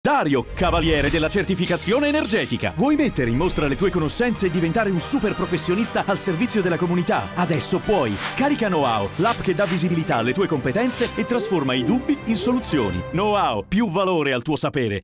Know How, l'app per le consulenze professionali
Hai già provato Know How? Se stai cercando un professionista, se hai bisogno di risolvere un problema o stai pensando di aprire una nuova attività, Know How è il posto giusto dove risolvere i tuoi dubbi.

Vieni a conoscere Dario, Cavaliere delle certificazione energetica!

Professionisti di tutta Italia a disposizione per ogni chiarimento di natura Legale, Economica, Tecnica o Scientifica.

Know How consente ad utenti e professionisti di avviare un primo approccio alla consulenza, superando i tradizionali ostacoli che solitamente rendono difficile la richiesta di supporto verso un esperto di settore.

Quali professioni su Know How? Scoprile tutte:

► AREA ECONOMICA: Commercialista, Tributarista, Consulente del Lavoro, Consulente Finanziario, Consulente Assicurativo, Consulente Aziendale, Consulente Marketing e Comunicazione;

► AREA LEGALE: Avvocato, Notaio, Dottore in Legge, Giurista d'impresa, Consulente Privacy;

► AREA TECNICA: Architetto, Geometra, Geologo, Consulente Immobiliare, Ingegnere.

► AREA SCIENTIFICA: Agronomo, Attuario, Biologo, Biologo Nutrizionista, Chimico, Consulente Informatico, Consulente Sicurezza sul lavoro, Enologo, Fisico, Gemmologo, Matematico, Statistico, Zootecnico.

CREDITS

Cast

Thomas Basilico
Antonio D'Emilio

Troupe
Regia Michele Saia
D.o.p ( director of photography ) Roberto Zazzara
Operatore Francesco Trisi
Costumi Marco Antonecchia
Trucco Sara Vitale
Montaggio Matteo Di Simone
Motion Graphc  Emiliano Colantoni Lino Fabrizio  Andrea Bellini

Stagisti IFA scuola di cinema
Ass. Regia  Lorenzo Aloisi
Ass. Operatore Giacomo Piccolo
Elettricista Filippo Longo
Elettricista Marco Mancinelli